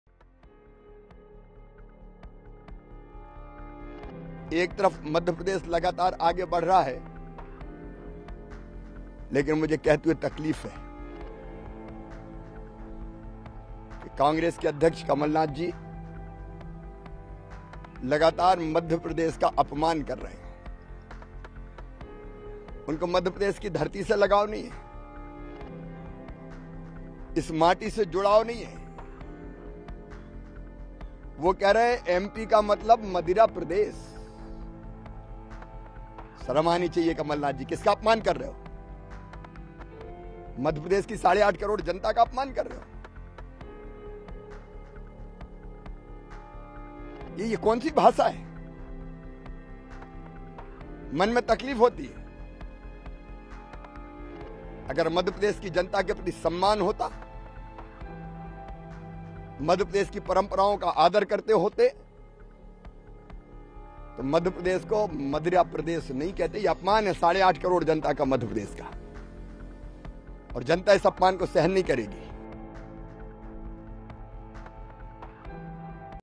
Kamalnath पर हमलावर हुए Shivraj Singh Chouhan | Dainik Bhaskar Hindi | दैनिक भास्कर हिंदी
कांग्रेस नेता कमलनाथ की 'मदिरा प्रदेश' टिप्पणी के जवाब में, मध्य प्रदेश के मुख्यमंत्री शिवराज सिंह चौहान ने कहा कि वह राज्य के लोगों का अपमान कर रहे हैं। “कांग्रेस नेता कमलनाथ कह रहे हैं कि एमपी का मतलब है ‘मदीरा प्रदेश’। उन्हें ऐसी भाषा का इस्तेमाल करने के लिए शर्म आनी चाहिए। वह राज्य के लोगों का अपमान कर रहे हैं और लोग इसे बर्दाश्त नहीं करेंगे, ”शिवराज सिंह चौहान ने कहा। कांग्रेस नेता और पूर्व सीएम कमलनाथ ने पहले राज्य की शराब नीति की आलोचना की और कहा कि मध्य प्रदेश अब मदिरा (शराब) प्रदेश बन गया है। उन्होंने यह भी आरोप लगाया कि शिवराज सरकार पूरे मध्य प्रदेश में सस्ती शराब बेच रही है, मोदी सरकार के यही अच्छे दिन हैं.